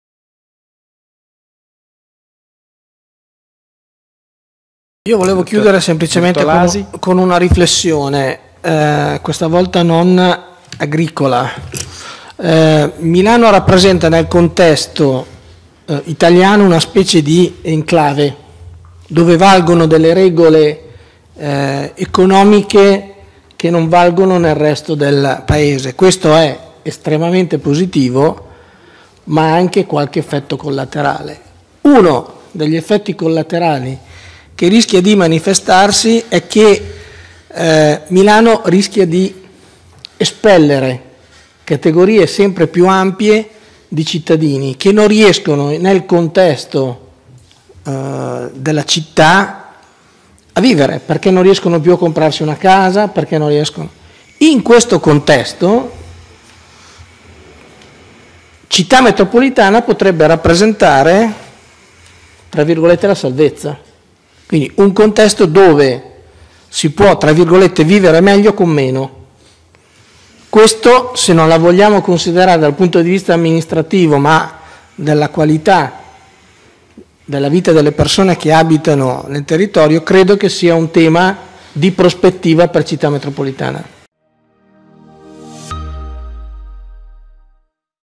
Seminario "What next"? 10 aprile 2024 - Secondo intervento di Umberto Bertolasi
Sono passati dieci anni da quando la legge 56/2014 istituì le Città metropolitane. In occasione del decennale della sua istituzione, la Città metropolitana di Milano organizza un seminario - articolato in più tavole rotonde che coinvolgeranno esperti, stakeholder e amministratori - non solo per riflettere sullo stato dell'arte del governo metropolitano, ma anche per avanzare proposte per il futuro.